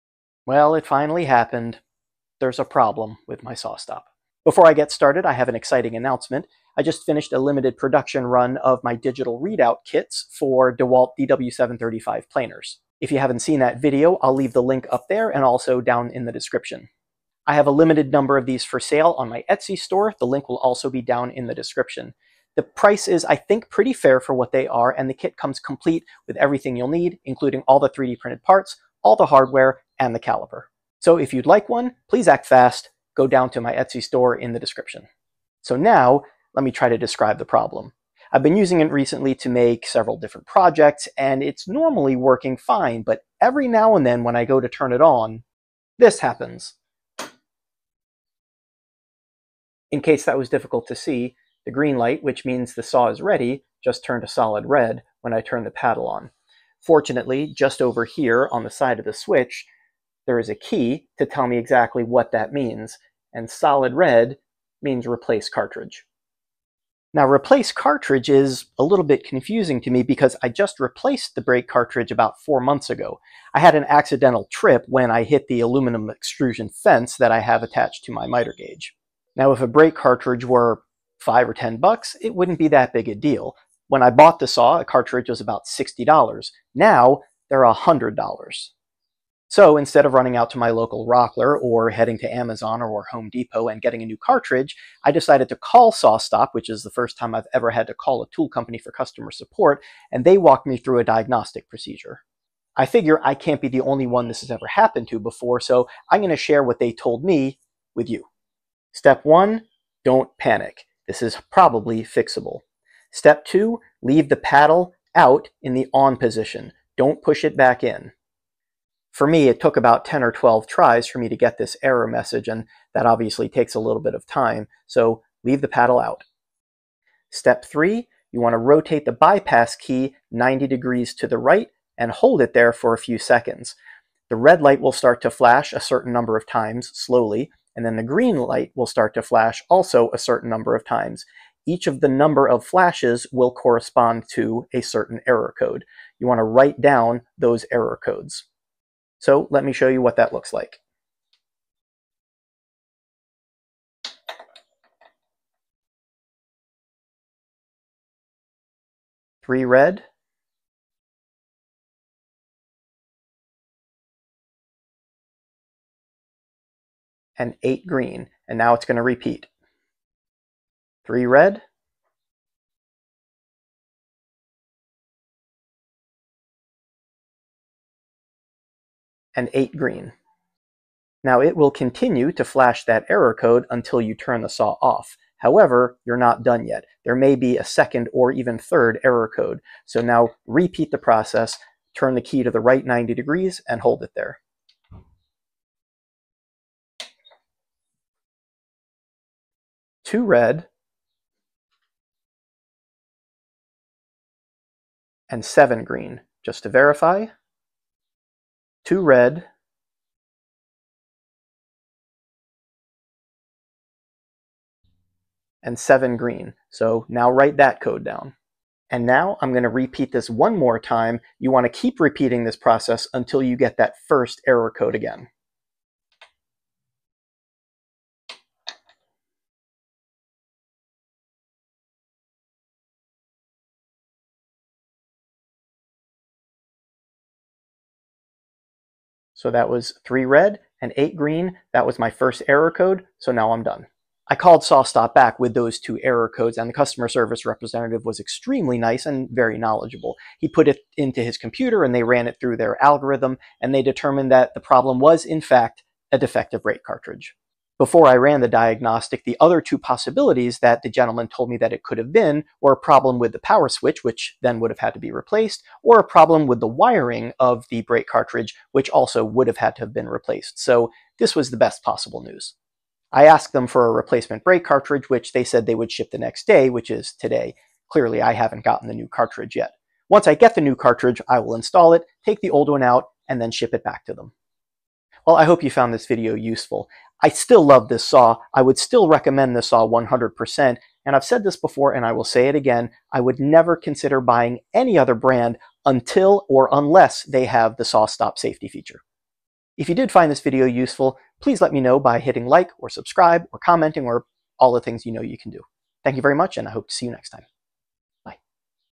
Solid RED! Sawstop error - how to diagnose an error code
Dewalt DW735 Digital Readout

My Sawstop is giving me an error code, so in this video I walk through the process of determining what the error is.

All my pieces are available for purchase in my store.